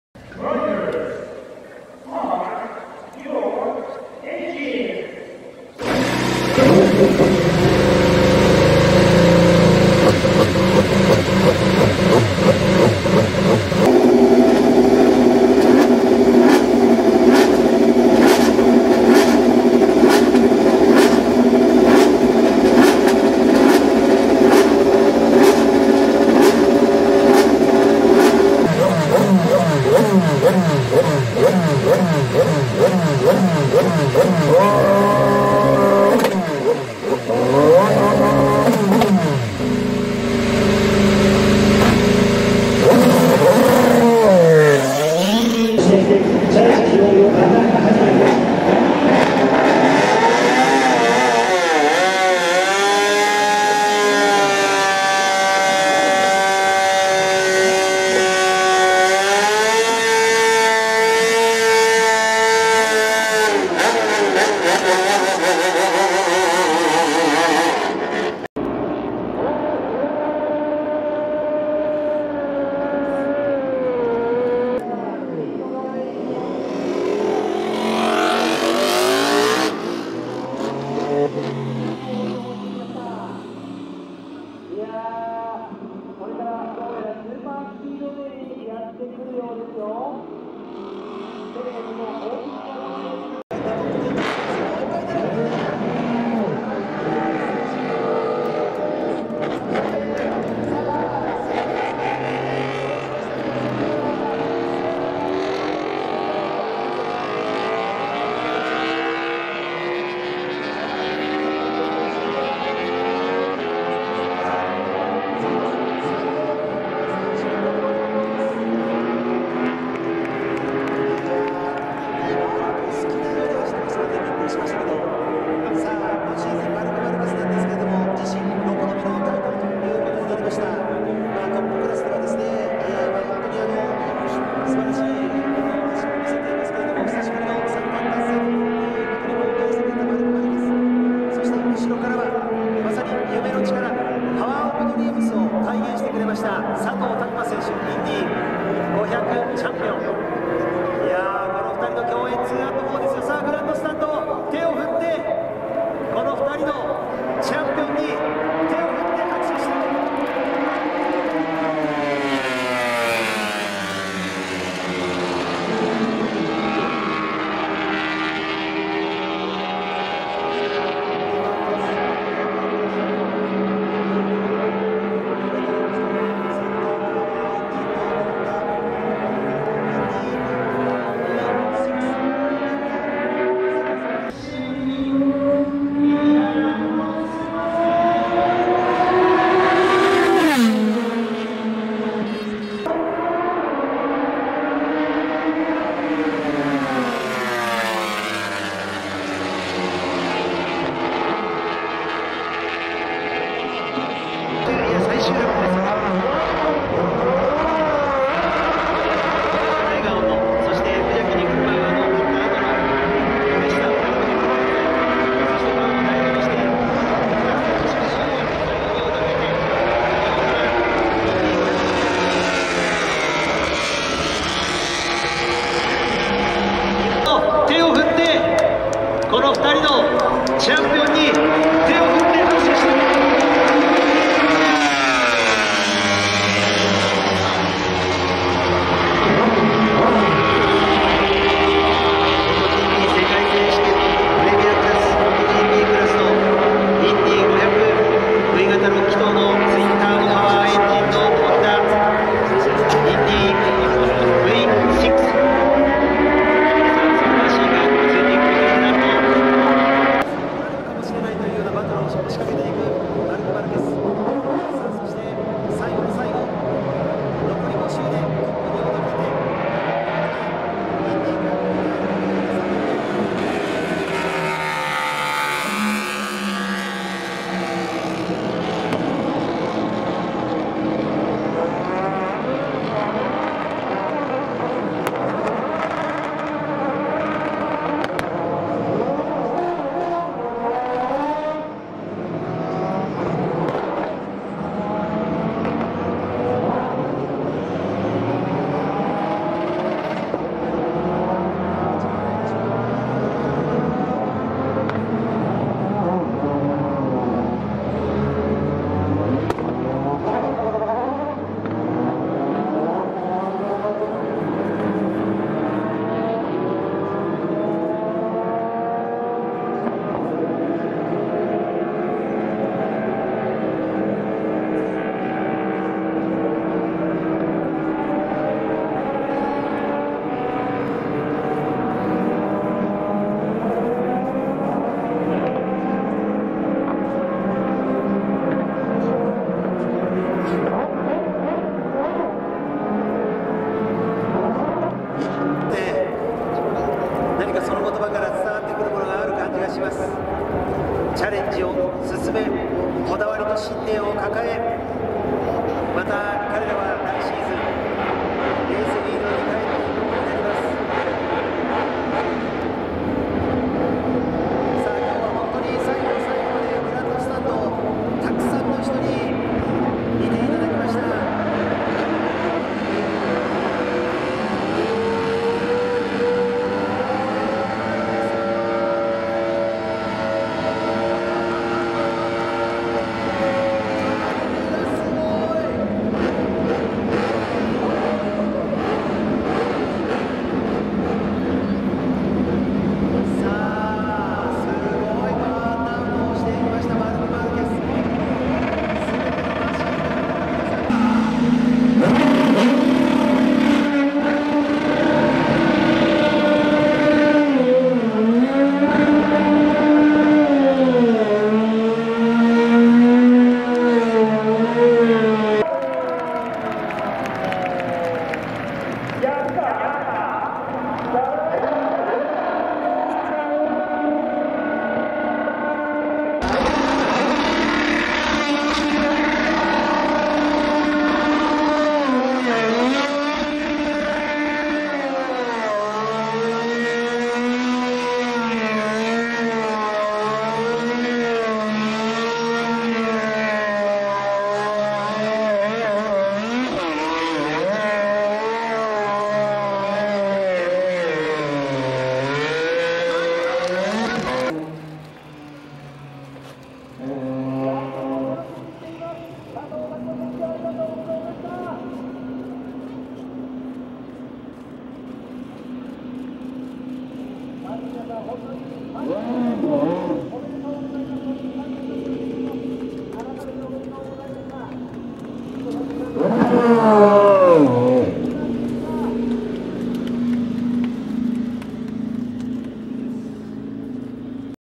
2017 Honda Racing Thanks Day - MotoGP™ vs. IndyCar
The Honda Racing Thanks Day is a traditional part of the Company’s racing year, bringing together official riders and drivers from Honda’s various motorsport programmes. The event marks the end of the racing season and is organized on the Motegi circuit by the world's leading manufacturer. This year 2017 MotoGP World Champion made a few laps with his RC213V vs Indianapolis 500 winner Takuma Sato, on a Honda-powered Dallara from the Andretti Autosport team. The Honda RC213V is powered by a 1000cc V-4 engine that produces approximately 247 horsepower, and weighs just over 350 lbs. It can accelerate from 0-186 mph in 11.8 seconds and reach a top speed of 225 mph. The Honda-powered Dallara IndyCar features a 2.2-liter twin turbo V-6's putting out estimated 550–750 horsepower depending on the level of boost used. They are limited to 12,000 rpm and are capable of reaching a top speed of 240 mph on the Indianapolis Motor Speedway oval layout.